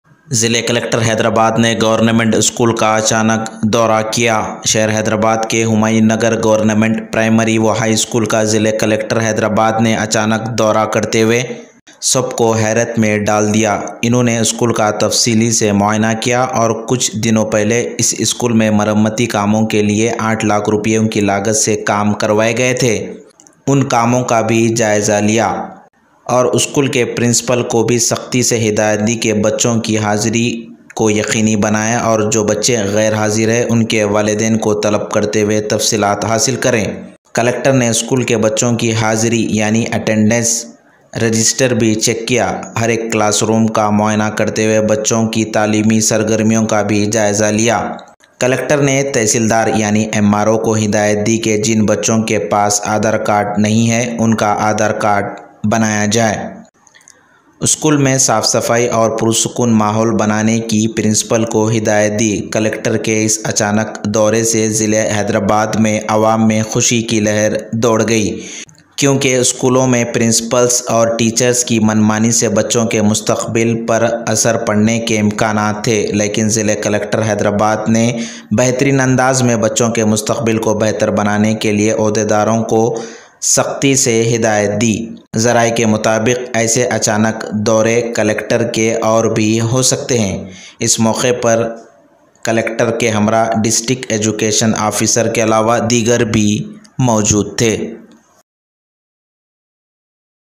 Hyderabad Collector Performs Surprise Visit at Government Primary and High School Humayun Nagar.
Hyderabad District Collector Performs Surprise Visit at Government Primary and High School Humayun Nagar | Independent Journalist | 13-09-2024.